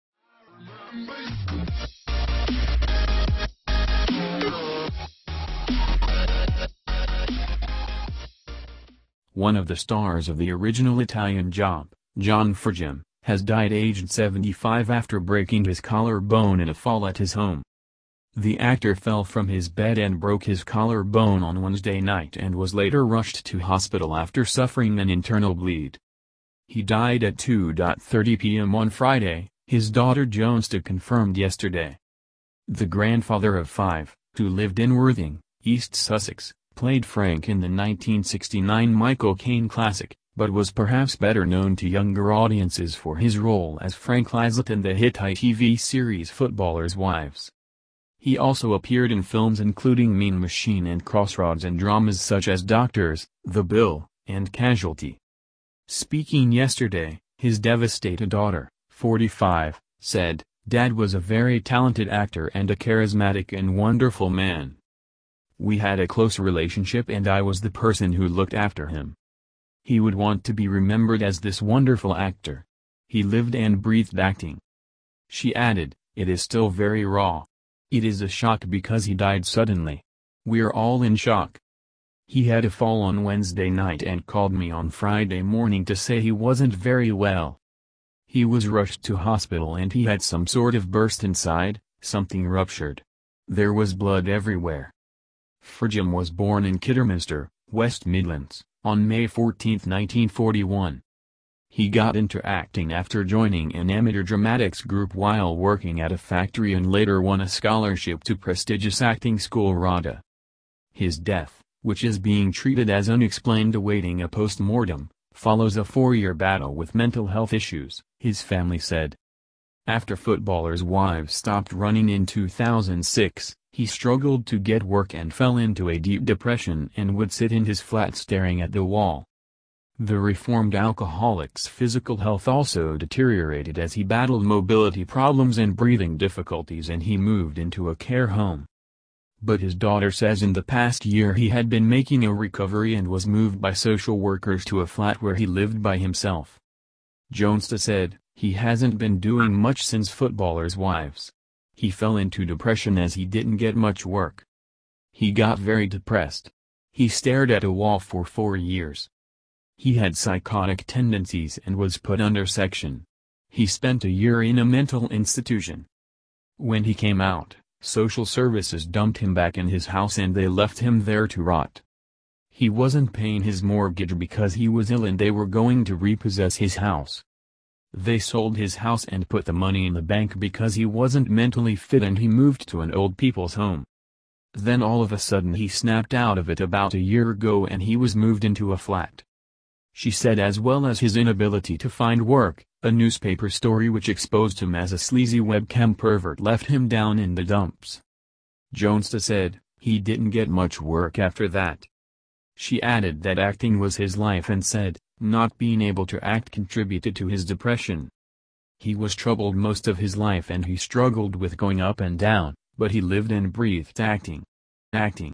Italian Job star John Forgeham dies aged 75 after suffering a fall at home
Italian Job star John Forgeham dies aged 75 after suffering a fall at home and breaking his collarbone
One of the stars of the original Italian Job, John Forgeham, has died aged 75 after breaking his collarbone in a fall at his home.